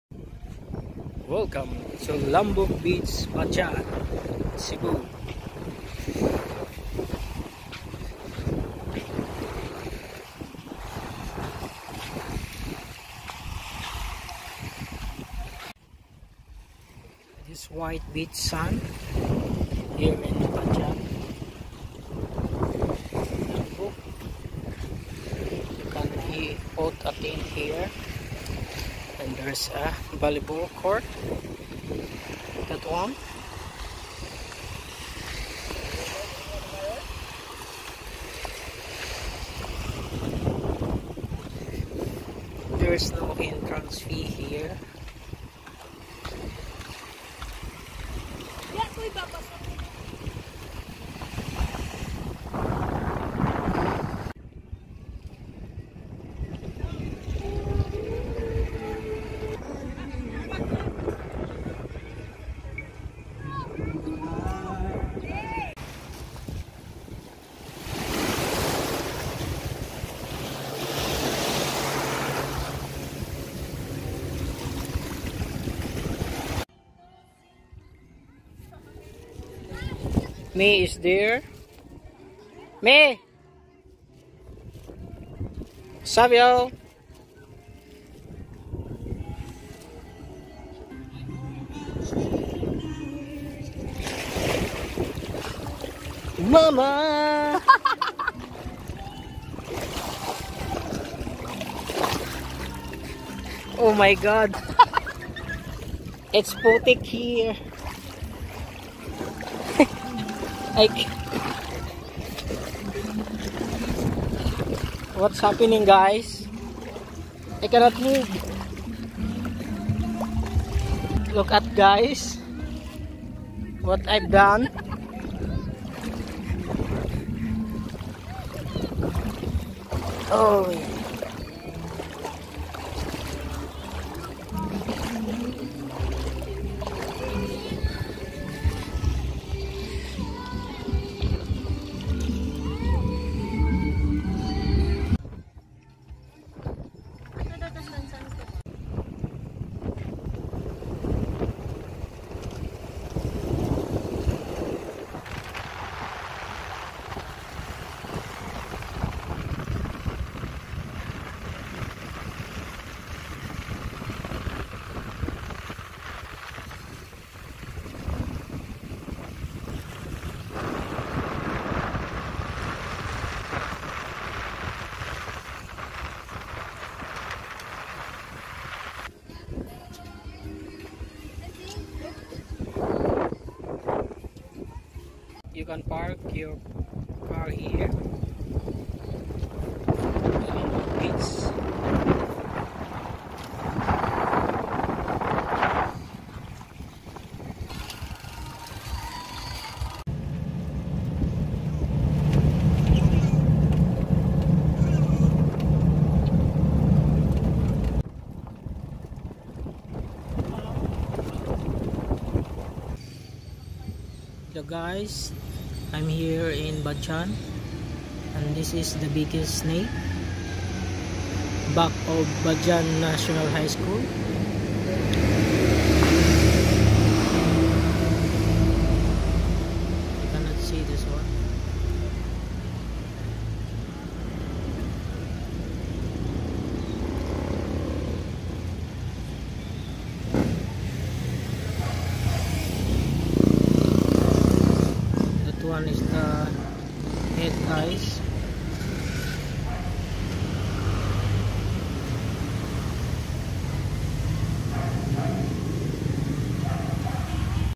Lambug White Beach in Badian Cebu
LAMBUG WHITE BEACH in Badian.TENTS ARE ALLOWED!

Let me give you a quick tour.